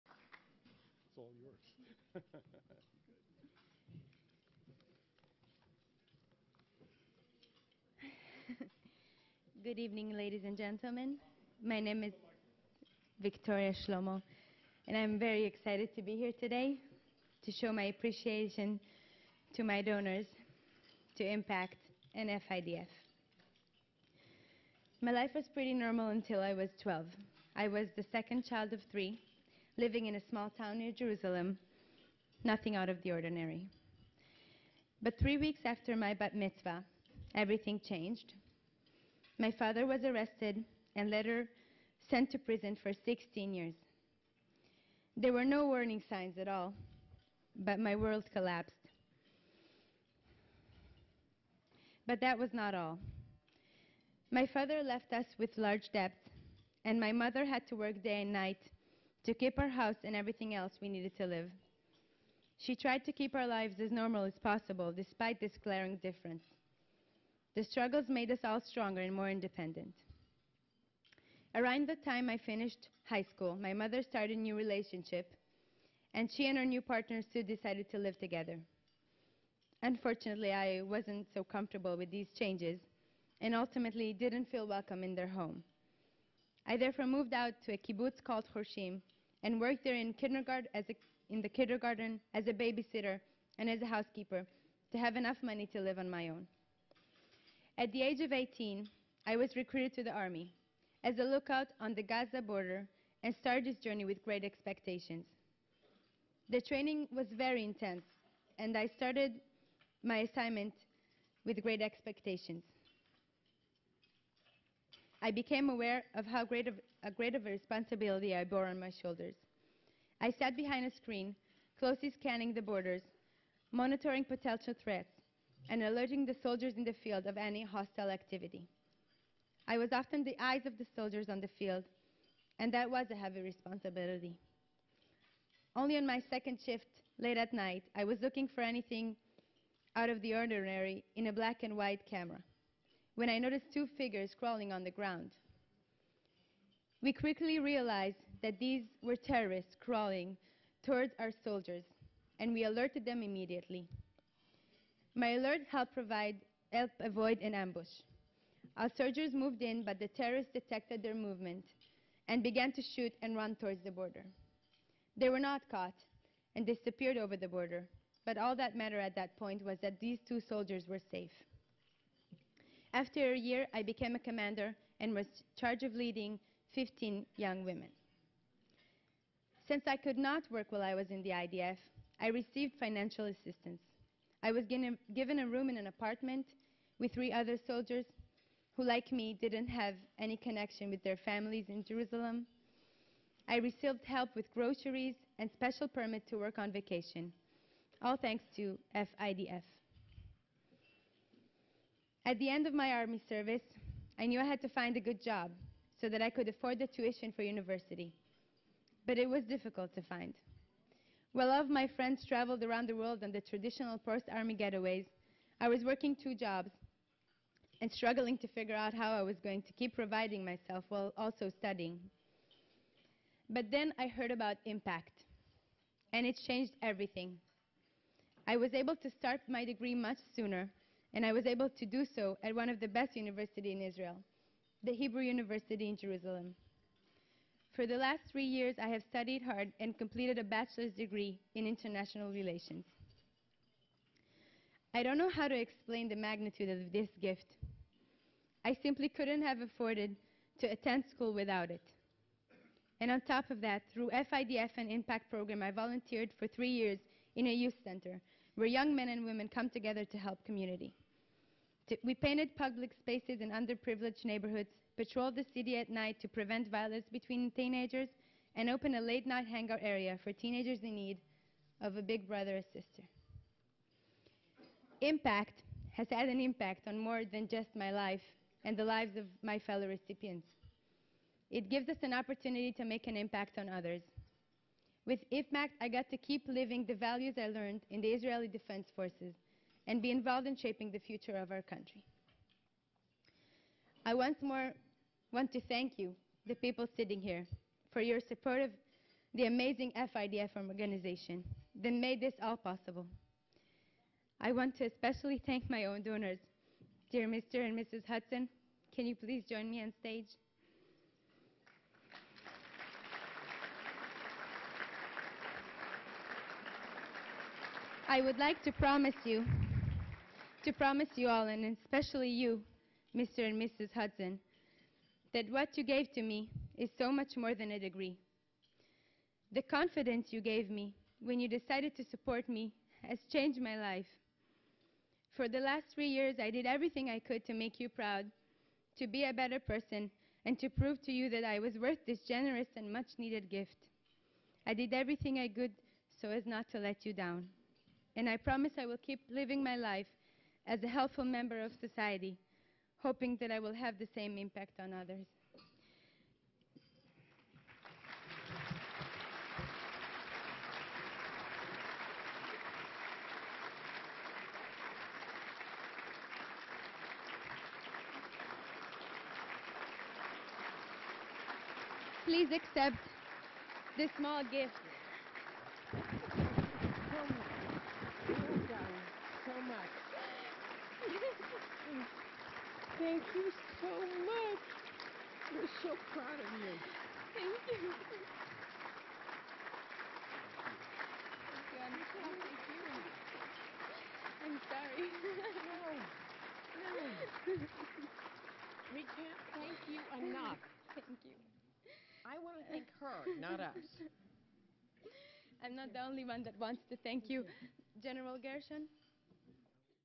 IMPACT! Scholarship Student Victoria Shlomo
The IMPACT! Scholarship Program helps Israel's veterans realize their dreams of a higher education b granting four-year scholarships to combat soldiers in financial need who have completed their military services. Dozens of Bay Area families have sponsored a student and thereby ensured a more educated population and stable future for Israel. Watch Impact student Victoria Shlomo show her appreciation to her sponsors at the 2013 San Francisco Gala Dinner.